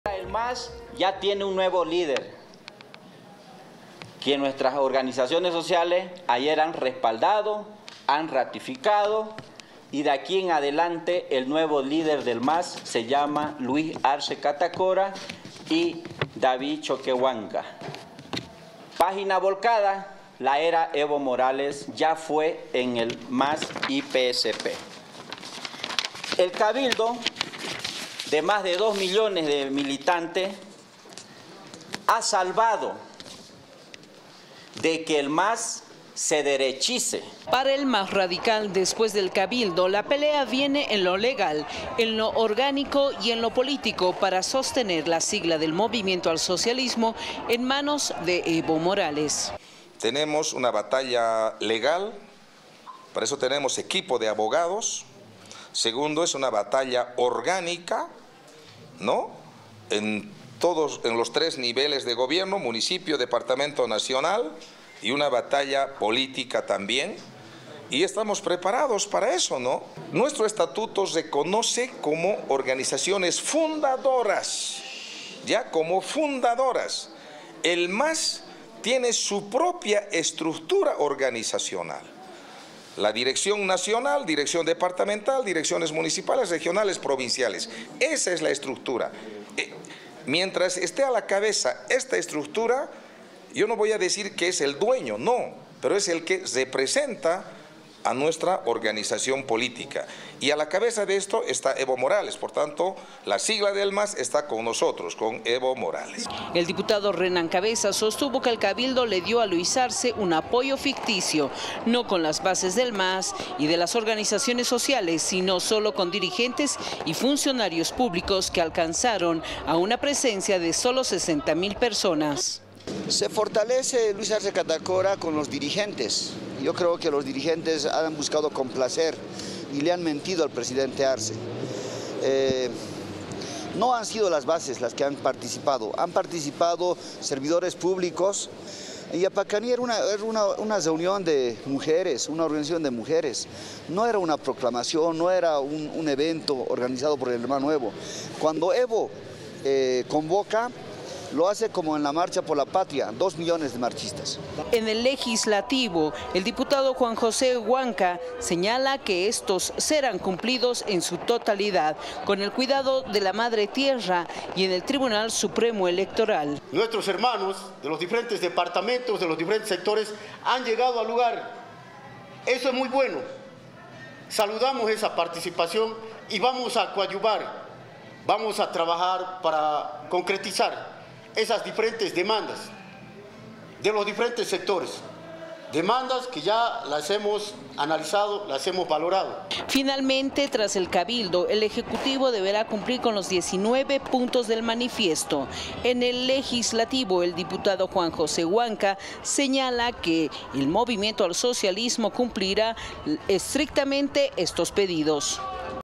“Arce nuevo líder del MAS y organizaciones”
Para el diputado Rolando Cuellar tras el Cabildo el nuevo líder del MAS y de las organizaciones sociales es Luís Arce, en los radicales aseguran que su apoyo fue ficticio.